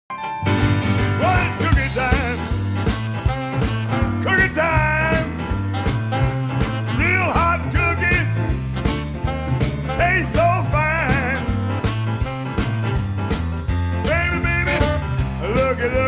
Cookie, sound added, Candy Animals 2, Paint by Number
Colored this cute cookie rabbit in the Candy Animals Collection in the Paint by Number app.  Sound added, check out other video for full image.